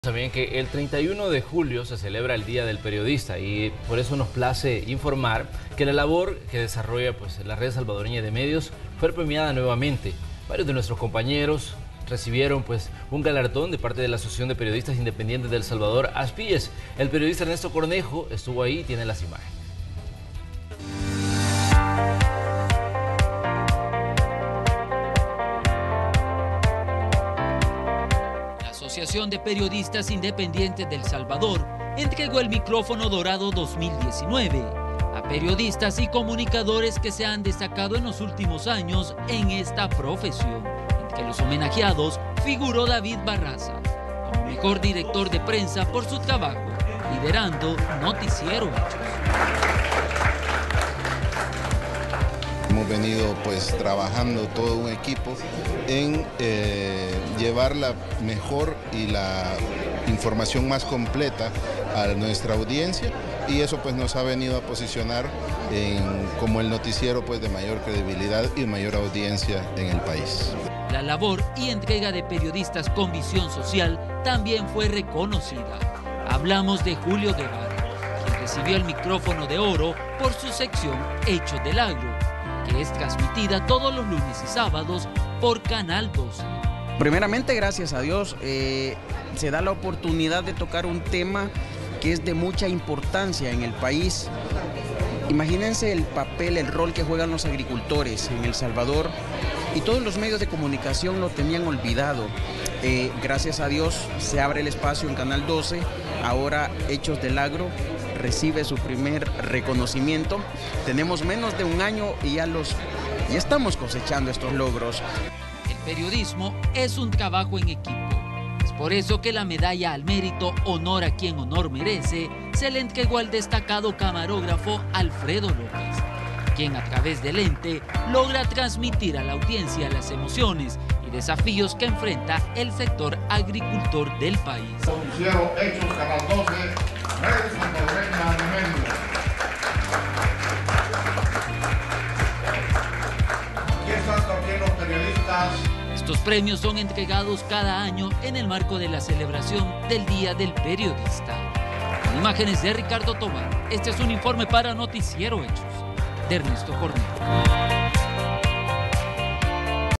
ASPIES premia a los periodistas más destacados del 2019
El 31 de julio se conmemora el ''Día del Periodista'', por lo que varios profesionales de las comunicaciones de Red Salvadoreña de Medios fueron destacados por ASPIES.